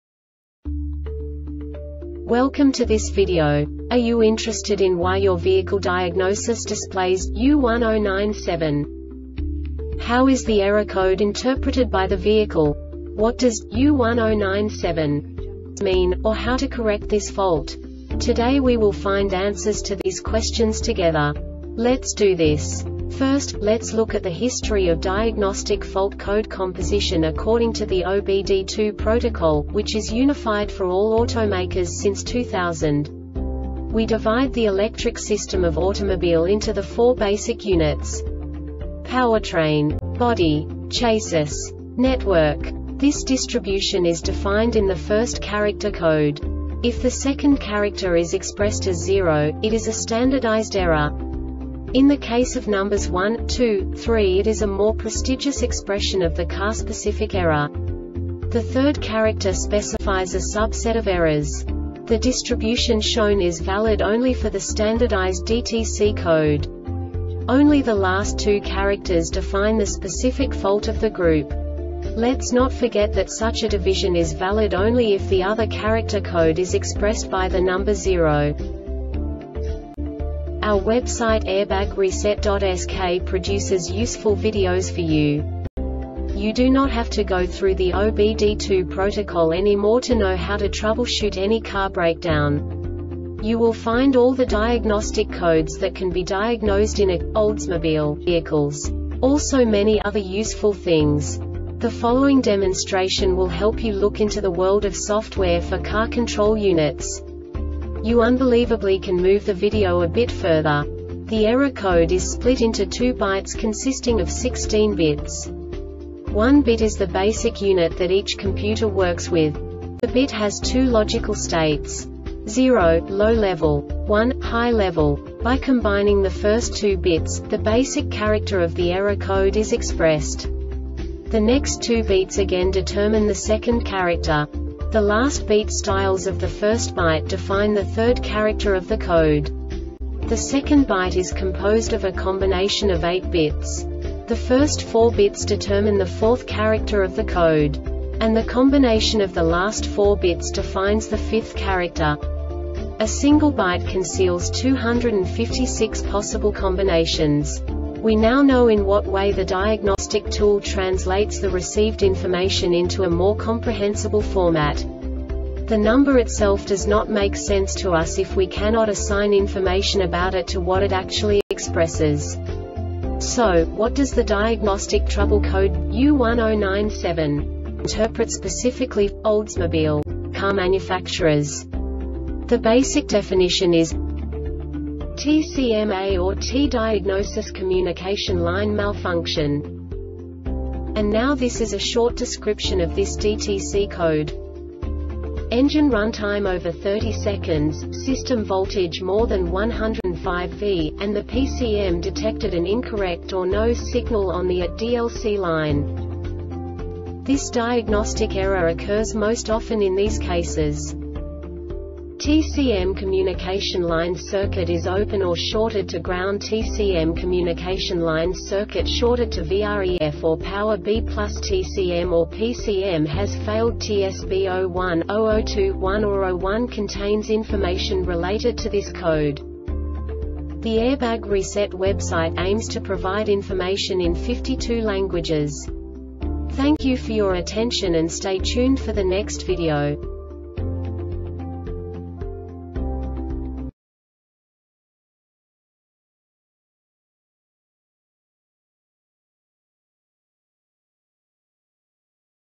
DTC Oldsmobile U1097 Short Explanation
Contents:
0:21 Basic DTC analysis according to OBD2 protocol standard.
1:48 Insight into programming
2:58 A diagnostically understandable description of the Diagnostic Trouble Code
3:05 Basic error definition

Make
Oldsmobile
Code
U1097
Definition
TCM A or T Diagnosis Communication Line Malfunction
Description
Engine runtime over 30 seconds,  system voltage more than 105v,  and the PCM detected an incorrect or no signal on the A or T DLC line
Cause
TCM communication line circuit is open or shorted to groundTCM communication line circuit shorted to VREF or power B+TCM or PCM has failedTSB 01-002 1 or 01 contains information related to this code